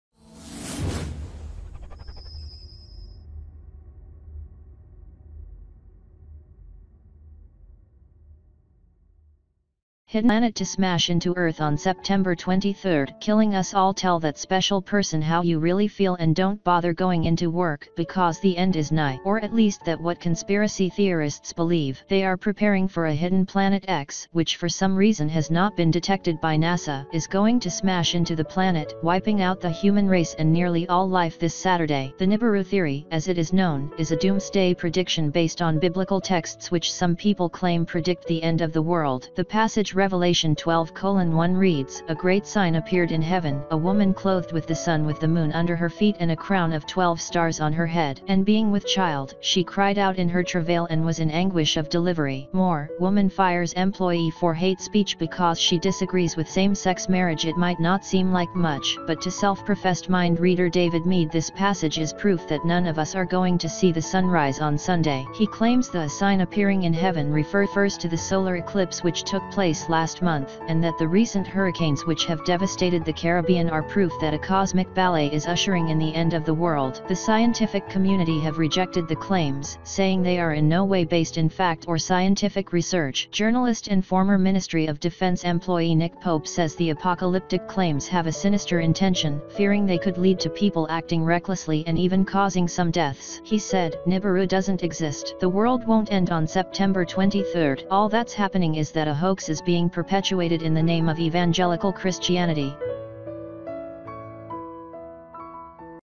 Hidden planet ‘to smash into Earth on September 23, killing us all. Latest News Aggregator
Hidden planet ‘to smash into Earth on September 23, killing us all
Tell that special person how you really feel and don’t bother going into work, because the end is nigh!Latest News Aggregator